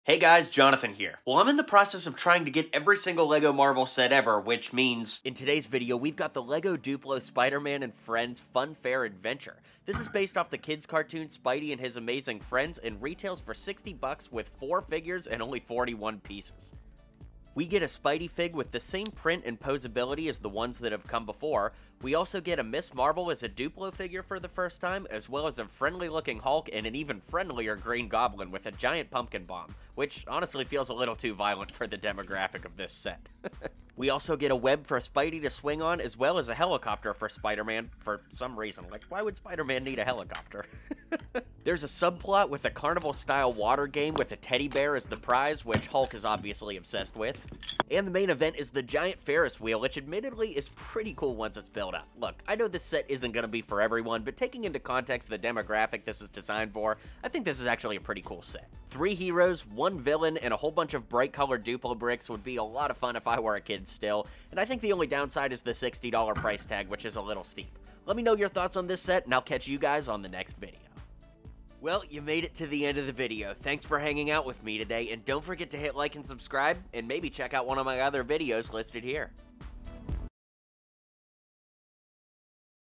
$60 LEGO Duplo Spider-Man Funfair Adventure Set 10963 Review
In this video, I'll show you the LEGO Duplo Spider-Man Funfair Adventure 10963 set with Spider-Man, Hulk, Green Goblin, and Ms Marvel!

Let me know what you think of this review of the LEGO video in the comments below, and let me know which characters you'd like to see me build next!